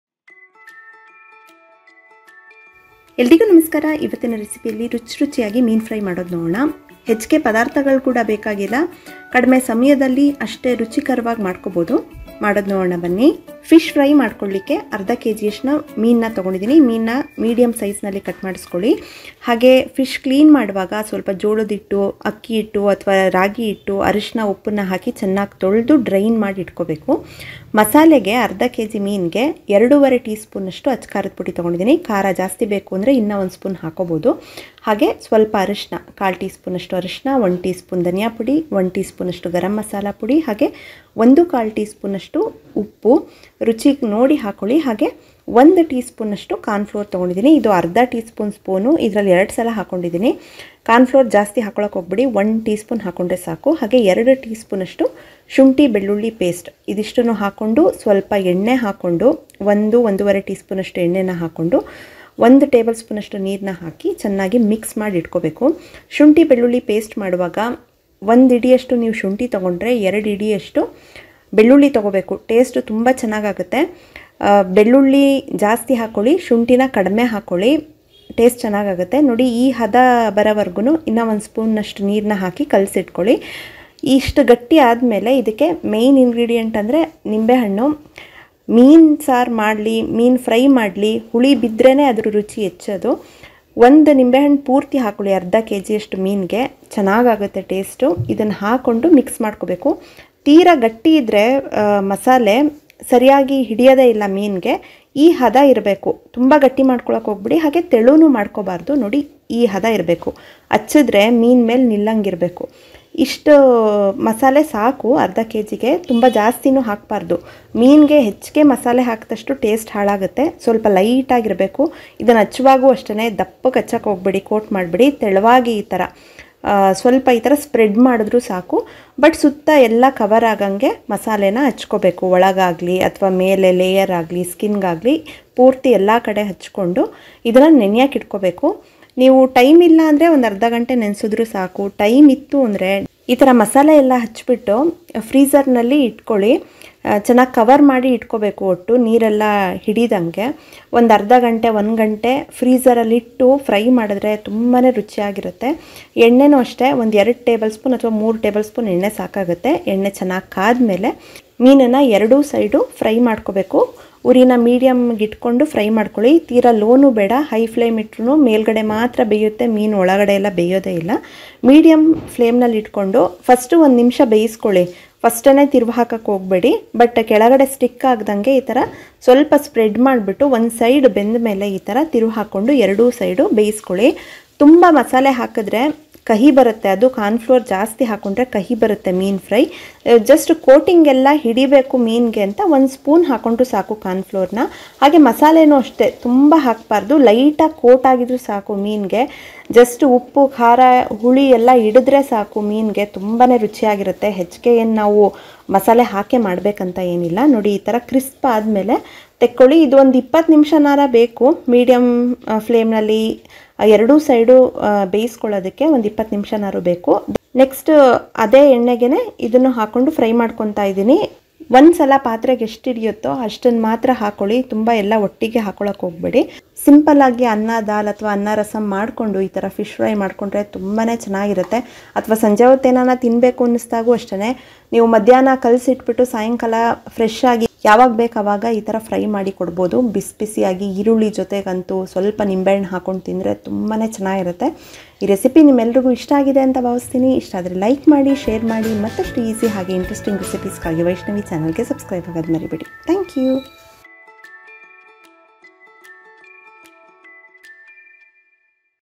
Fishfry / ಫಿಶ್ ಫ್ರೈ / Simple and tasty Fishfry
Ingredients :
Fish-1/2kg
chilli powder-2.5tsp
haldi-1/4tsp
coriander powder-1tsp
garam masala-1tsp
salt-1.25tsp
ginger garlic paste-2tsp
cornflour-1tsp
oil-3tbsp
water-2tbsp